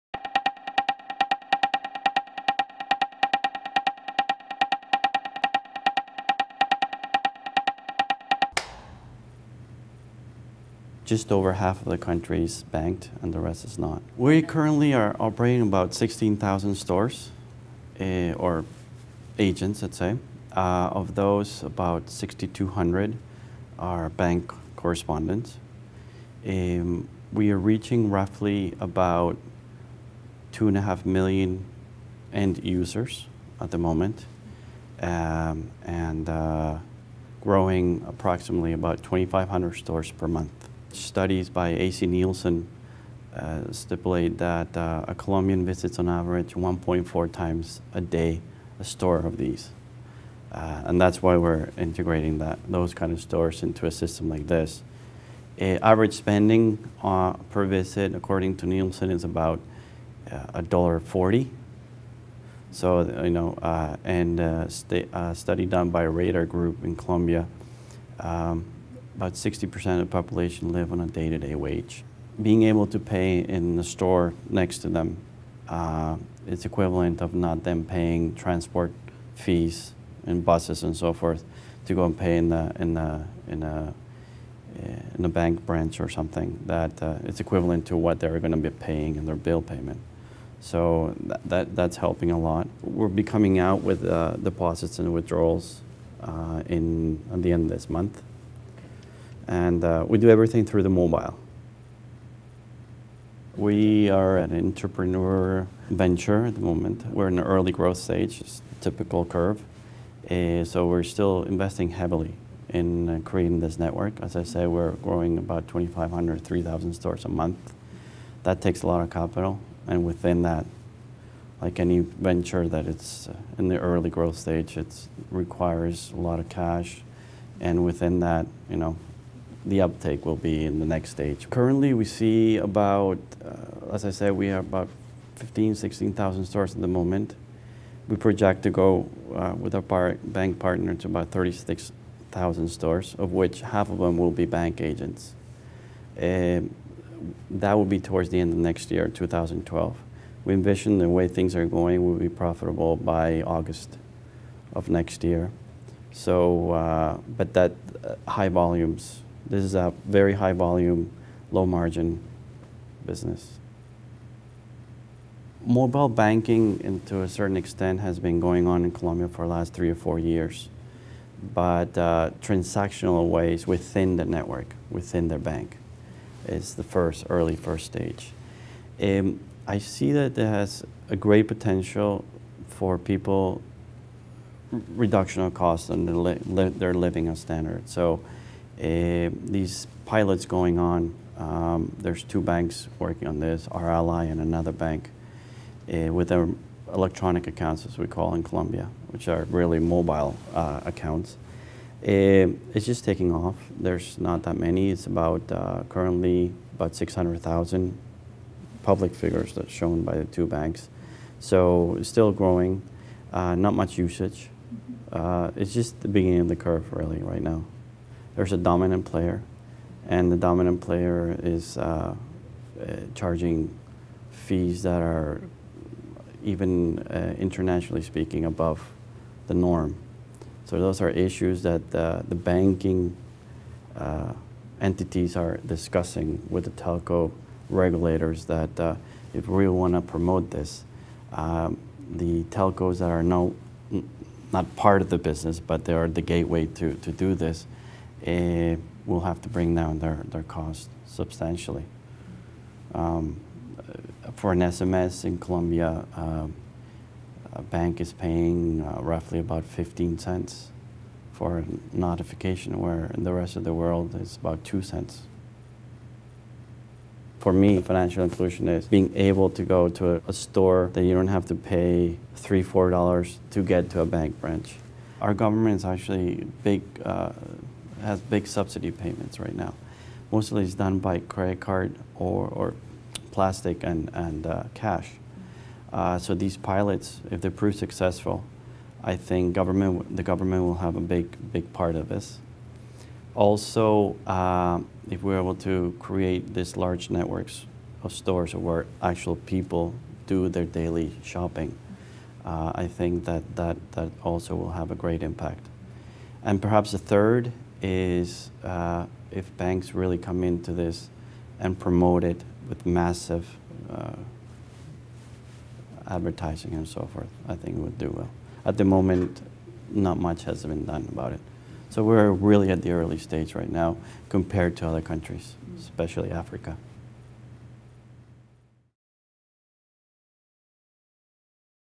Microfinance Now: Johannes Kling
In this short interview Johannes Kling of DD-DEDO describes the state of branchless banking in Colombia. Mobile banking has great potential for reducing the cost of financial services for low income consumers, says Kling. But with mobile and agent banking in operation for just a few years, the business is at an early stage of development in Colombia and it remains far behind other countries, especially African countries. According to Kling, for mobile and agent banking to take off the Colombian government will likely play a big part in using the networks for government payments. A number of other key ingredients will also be needed to make mobile and agent banking a success in Colombia: a large network of convenient local stores providing the services, lower fees from the telecom companies, and banks promoting the service.
Camera Peter Cairns. Produced by Jeanette Thomas.